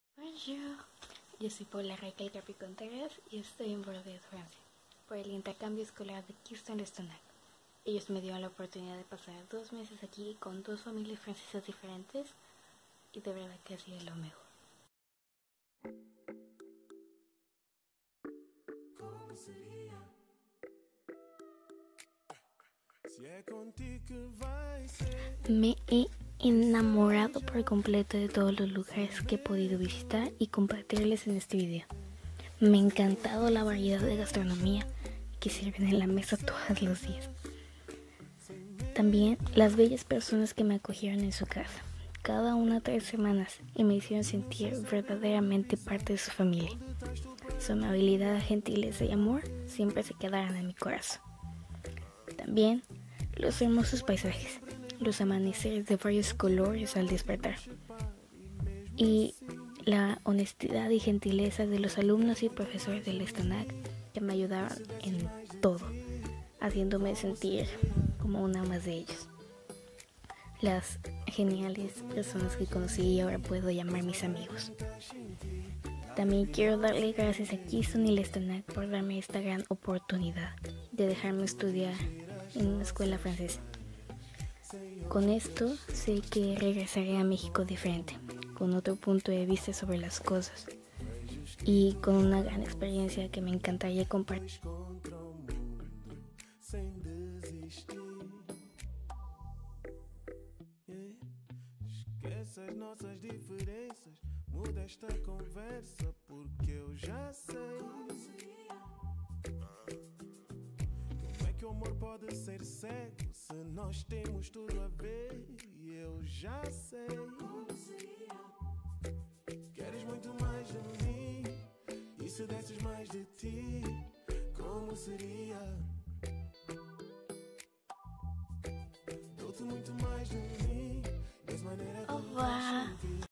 Testimonio del programa 6 semanas en Francia
Programa de Inmersión con el colegio Lestonnac Bordeaux France.
¿ y tú que estas en secundaria no te gustaría ir ?
Informes en Keystone International Education